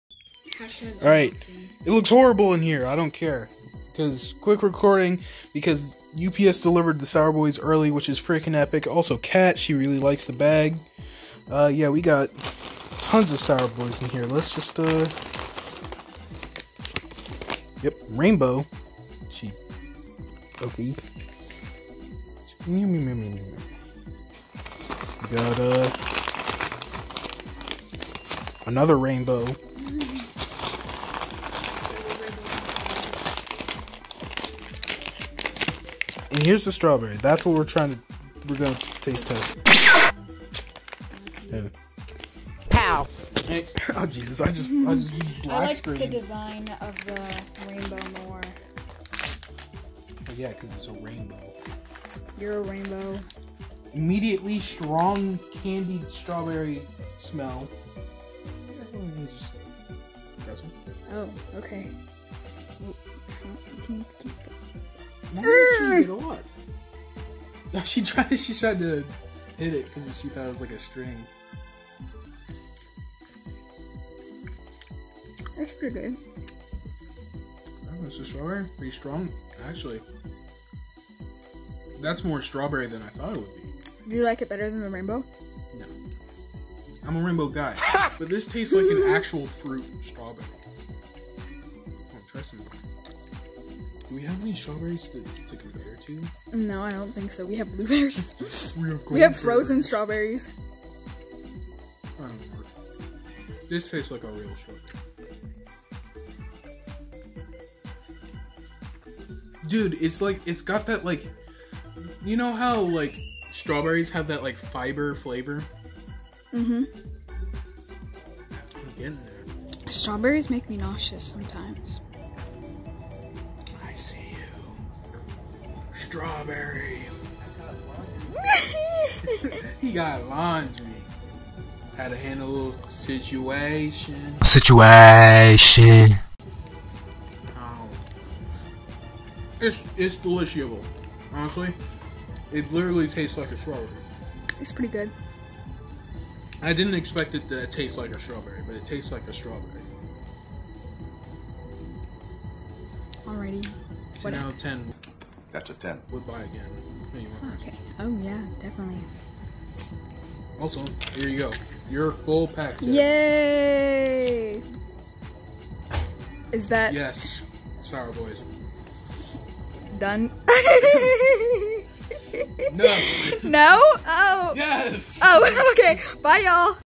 Trying Oompaville and Rifty's STRAWBERRY Sour Boys!
I ended up editing something I didn't originally want to. Oh well, I like the result.

sour.gg